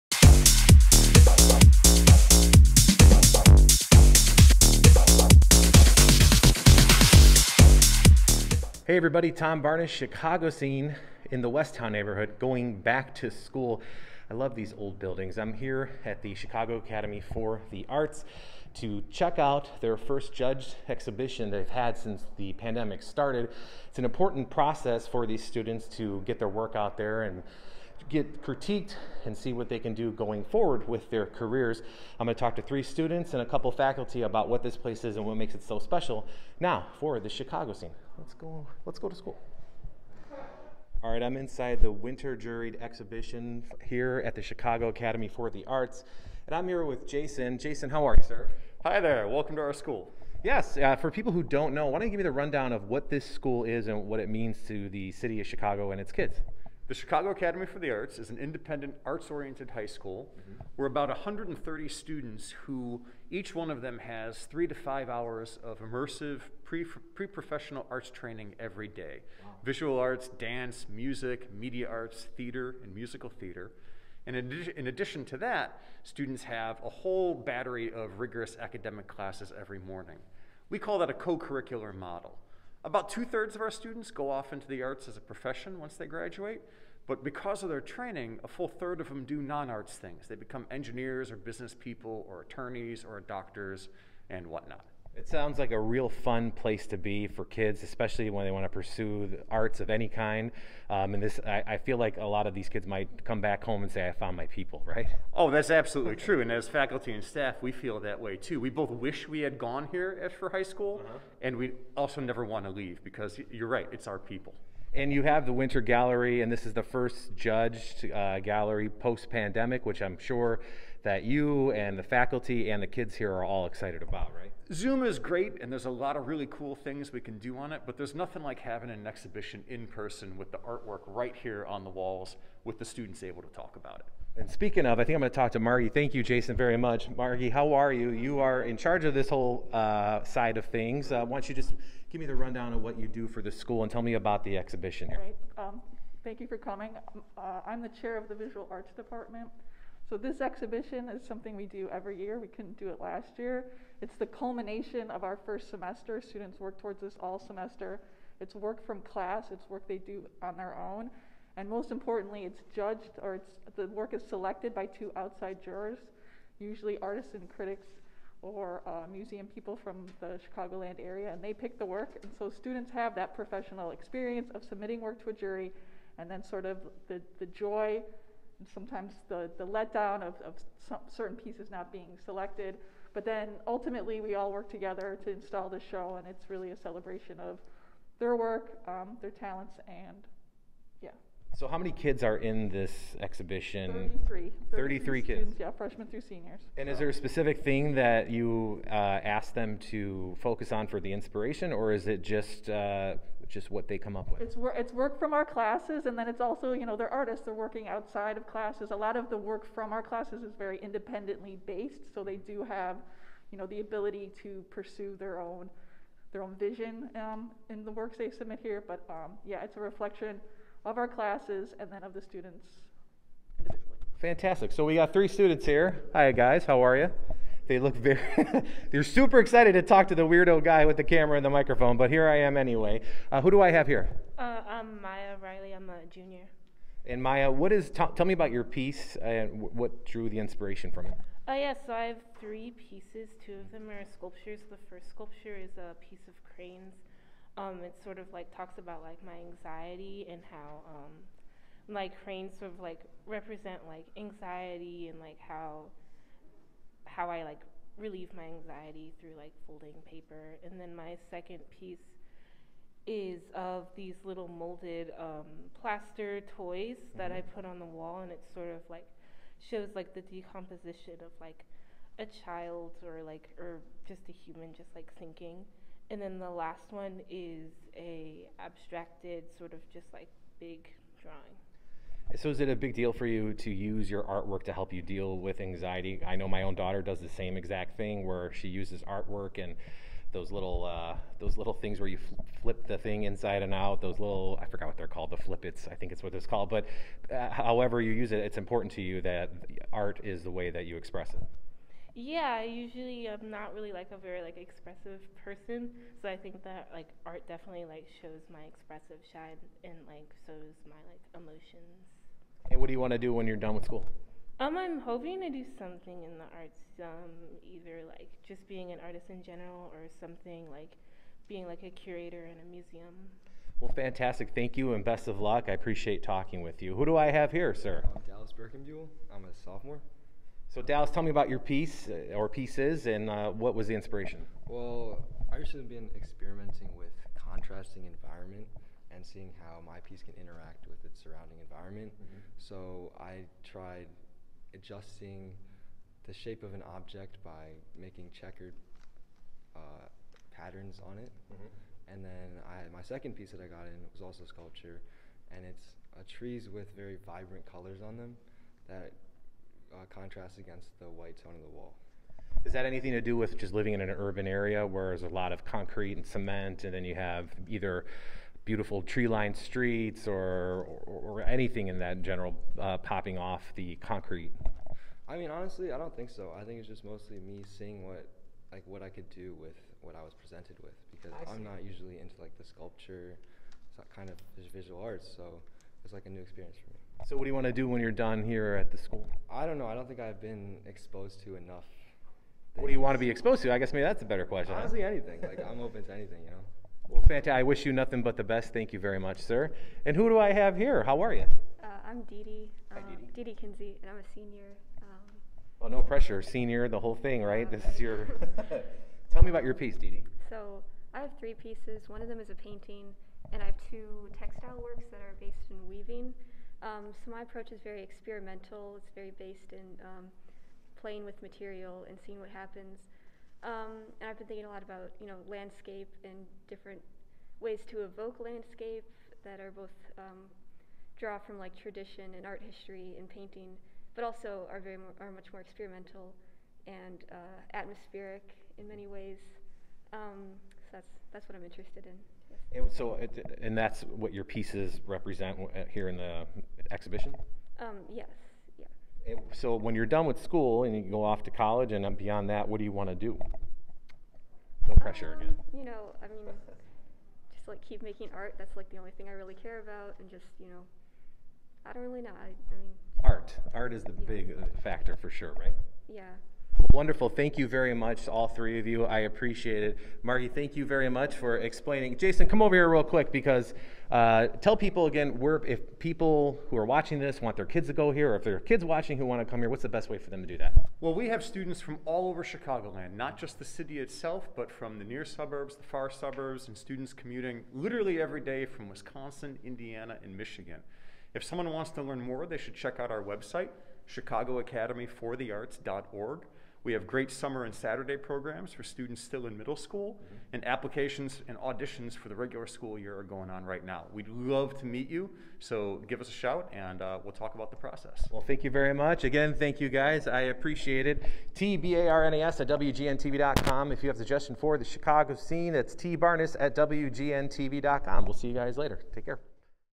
Story from the 78: Since the pandemic, Chicago Academy for the Arts students off their artistry
WEST TOWN
Students showcase their artistry through inventive, original works at the first in-person exhibit since the beginning of the pandemic at The Chicago Academy for the Arts.

The exhibit gave students’ their first opportunity in nearly two years to display their work live.

These showcase opportunities is what allows The Chicago Academy for the Arts to stand out as a beacon of opportunity as the school reinvents what a classroom has the capability to be.

“The students’ creativity amazes me each and every year”, says Margy Stover, Visual Arts Department Chair. “I’m proud of their dedication to expanding their artistic horizons and invited the public to enjoy their work.”

The Chicago Academy for the Arts is a nationally recognized independent high school for the performing and visual arts. The academy offers students the opportunity to engage in a unique co-curricular program that includes college-preparatory academic classes and professional-level arts training.

For more information on the Academy, click here.

For more Chicago Scene fun, follow @tomwgnchicago on Instagram.